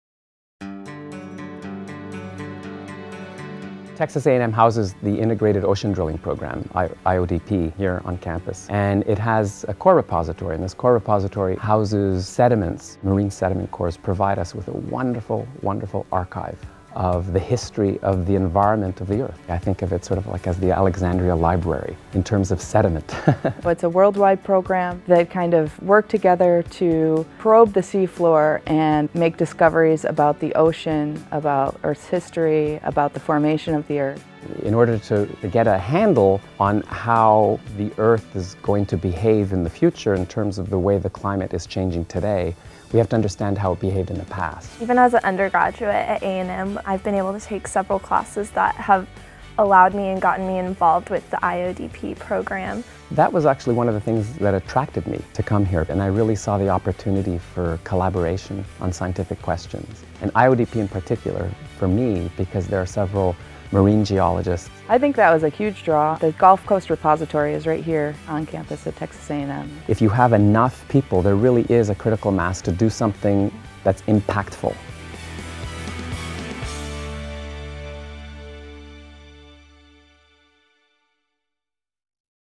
International Ocean Discovery Program (IODP) at Texas A&M
Texas A&M's Integrated Ocean Drilling Program (IODP) is part of the world's largest, longest-running, and most successful international scientific research drilling program. IODP offers students and faculty-researchers a unique opportunity to participate in meaningful international research that offers hands-on learning and research opportunities.